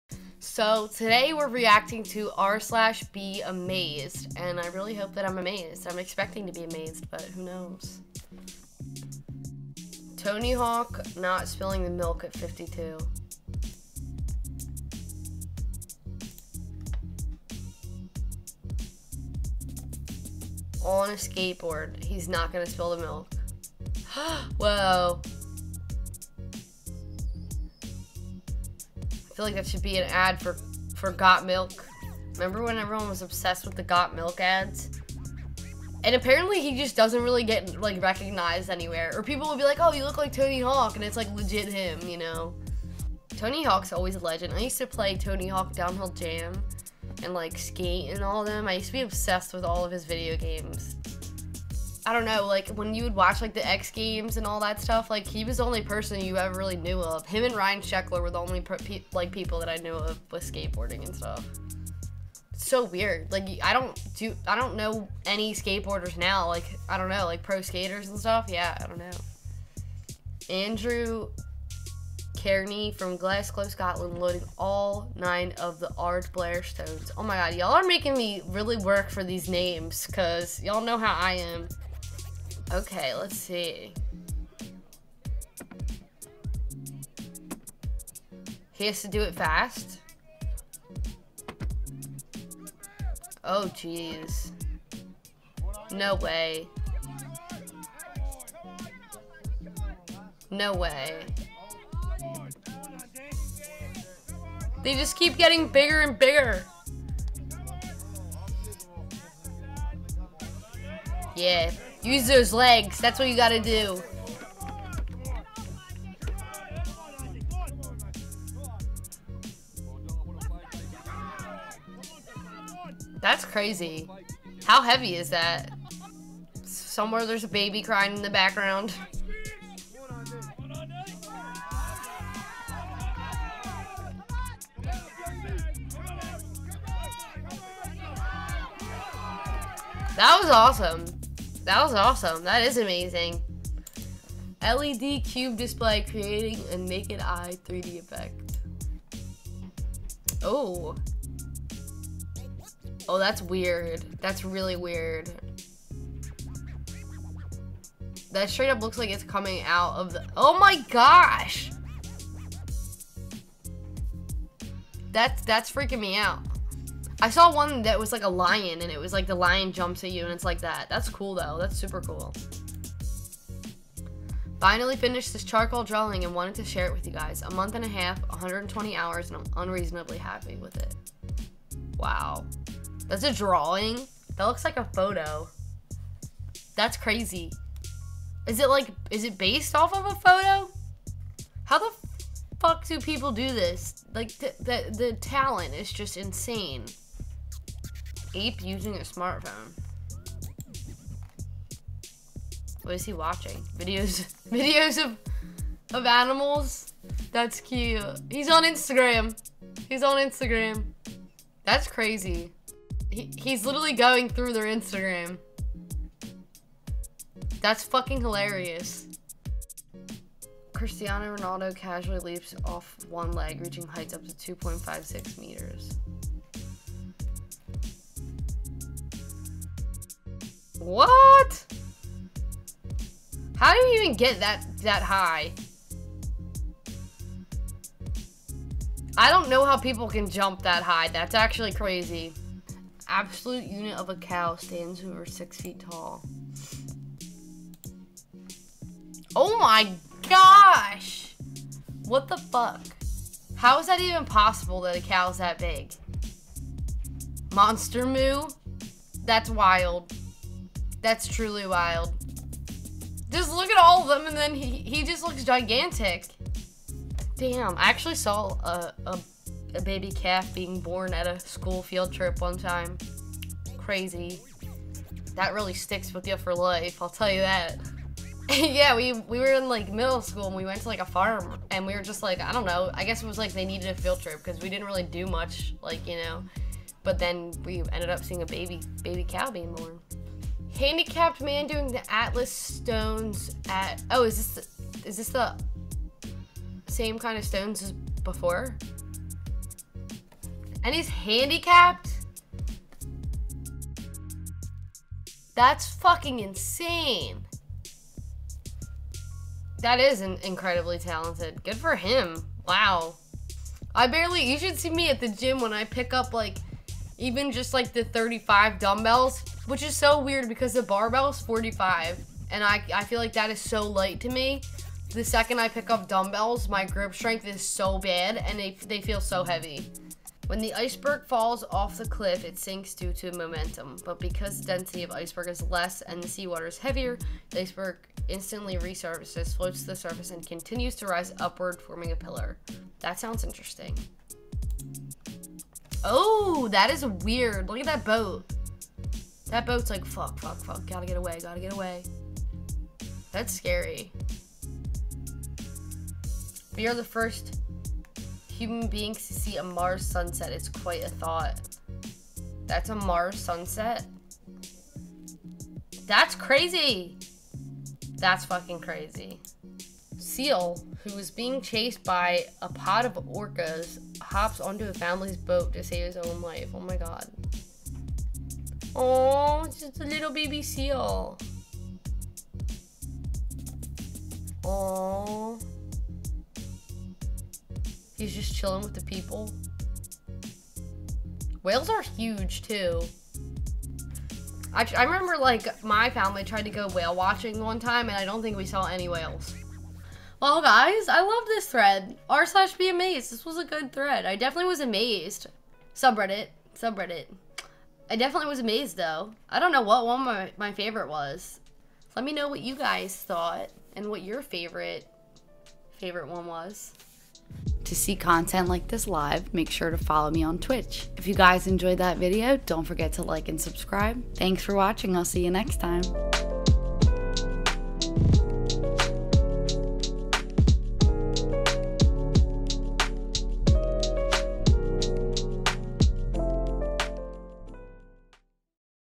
Reddit Posts That Butter My Biscuit
Looking at the top posts on Reddit's r/BeAmazed

My Gaming PC (ad)